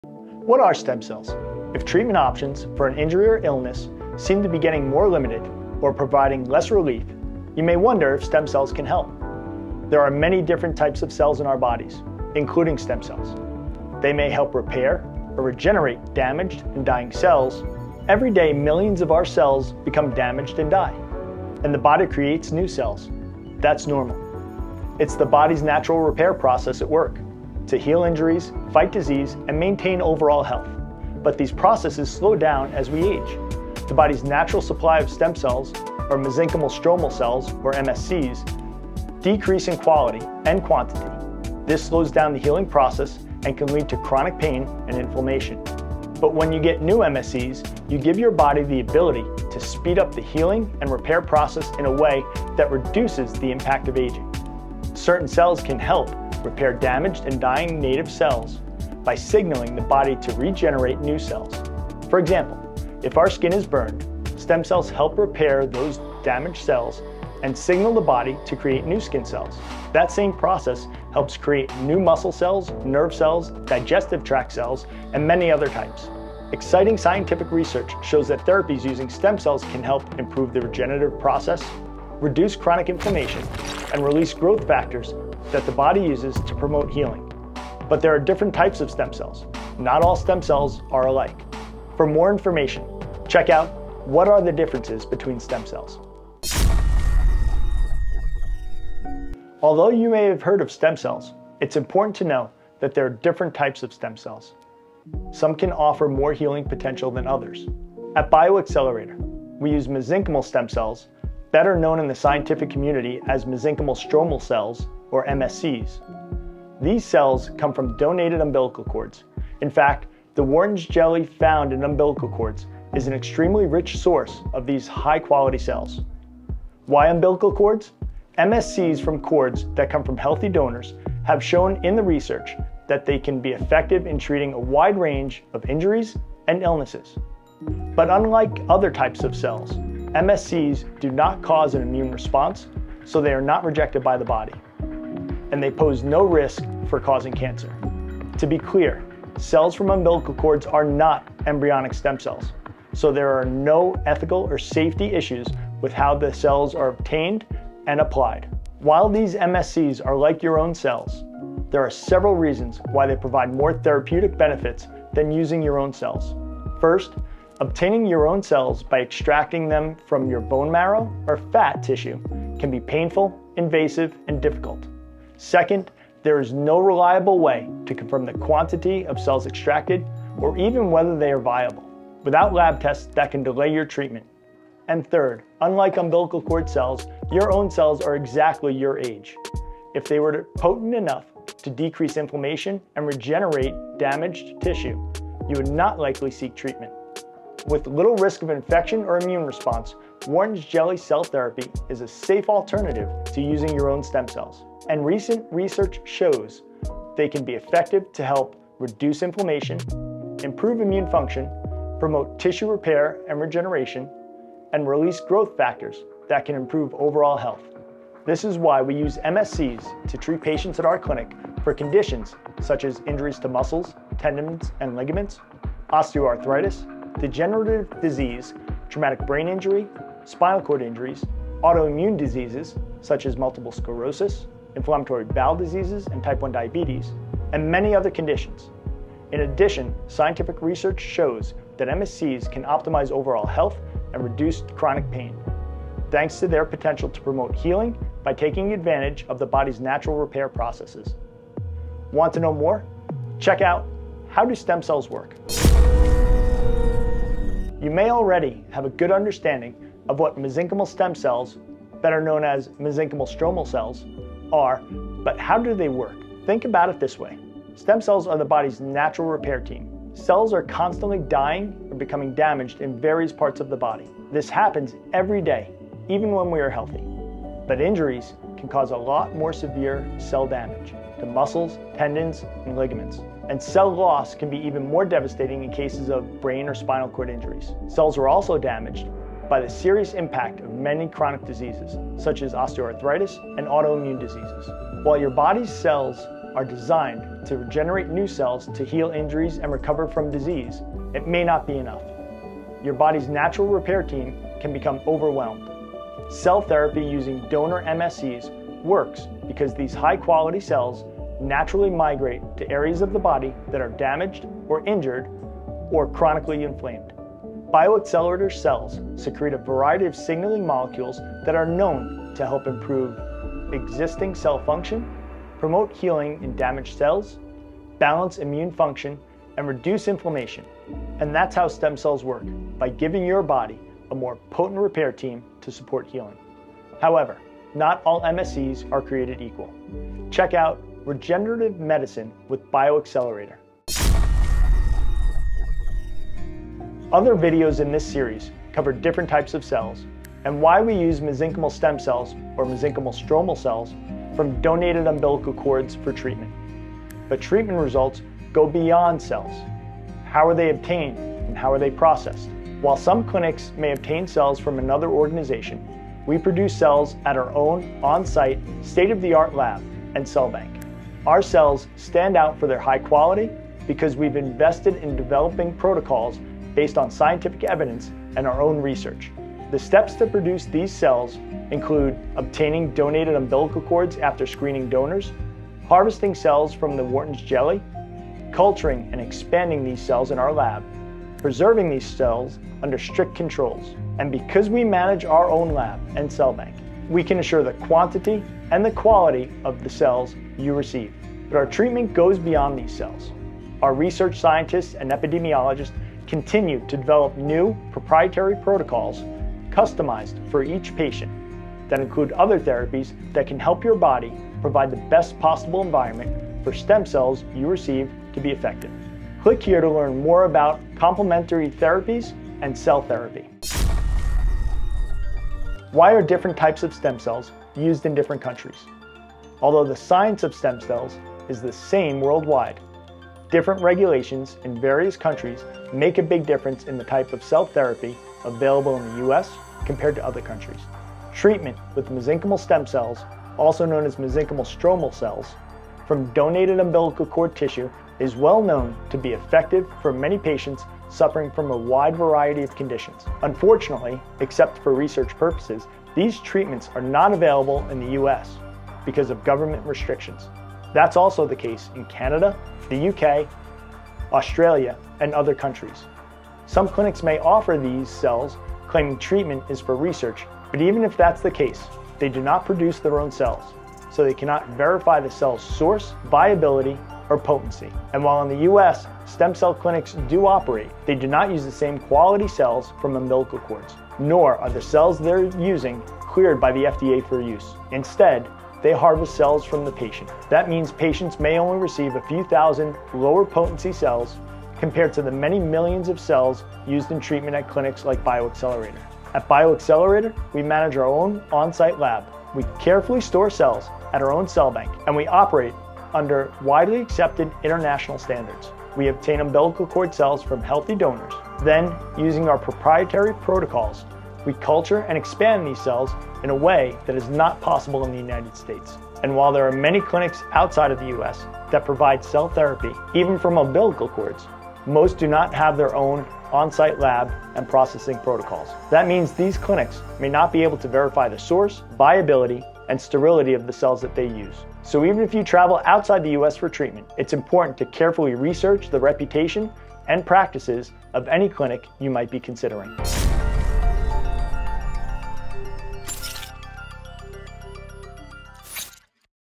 Discover the Astonishing Secrets of Stem Cells: Unveiling the Future of Medicine!
00:00 - 01:39 - WHAT ARE STEM CELLS?

01:40 - 04:17 -WHAT ARE THE DIFFERENCES BETWEEN STEM CELLS?

04:18 - 05:46  - HOW DO STEM CELLS WORK?

05:47 - 07:05 - REGENERATIVE MEDICINE WITH BIOXCELLERATOR CELLS

7:05 - 09:09 - WHY ARE DIFFERENT TYPES OF STEM CELLS USED IN DIFFERENT COUNTRIES?